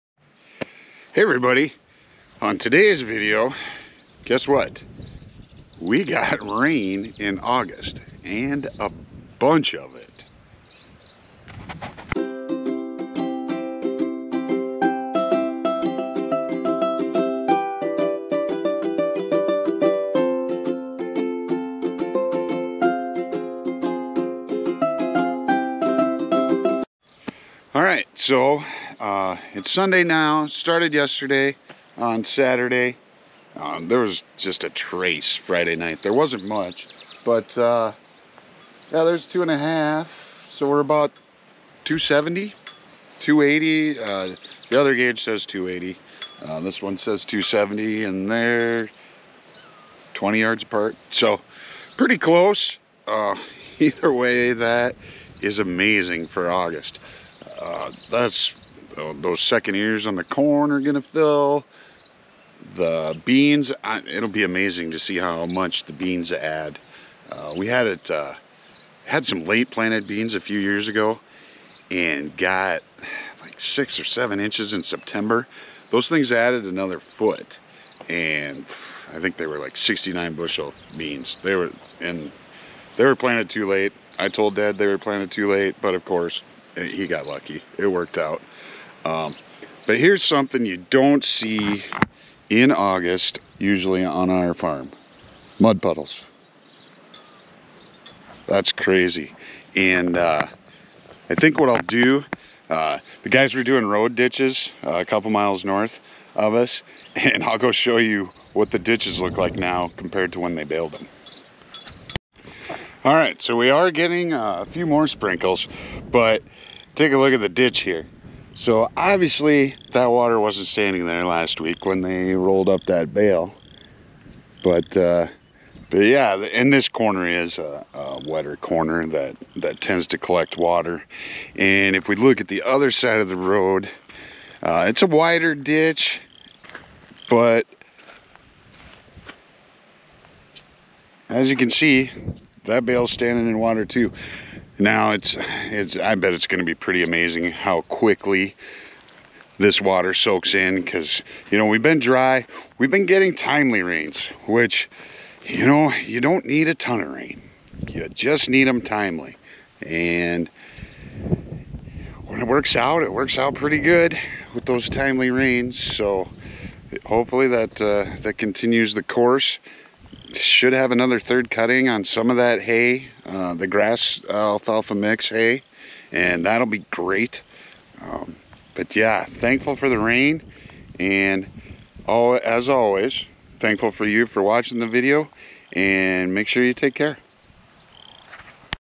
RAIN in August in South Dakota!!
2.80" inches of rain in Plano South Dakota